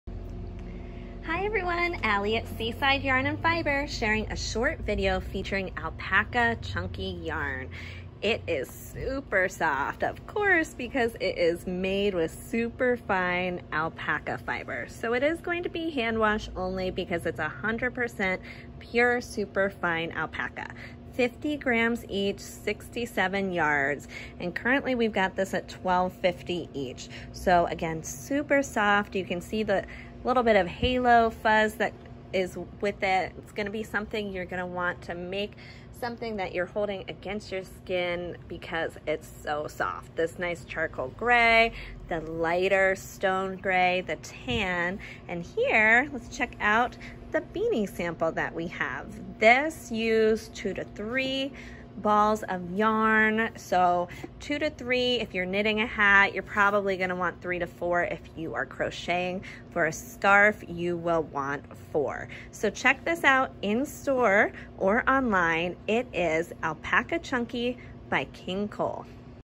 Alpaca Chunky Yarn by King Cole
This sumptuous natural Alpaca yarn comes in soft neutral shades, Fawn, Camel, Charcoal and Grey. Supported by a beautiful book full patterns from Socks, Hats & Scarves to Vest tops and shawls. With most projects only requiring 1 to 2 balls, this book is a fantastic cost-effective way of adding some luxurious Natural fibre to your wardrobe.

68 yds / 62
50 g

Recommended Needle size: US 10 / 6 mm

PATTERN IDEAS:
*Celtique Scarf by Collete Audrey; 3-5 balls / Knit
*Boars Hunt Capelet by Tina Baker; 4-6 balls / Knit
*Belovd Aran Hat by Solenn Couix-Loarer; 2-4 balls / Knit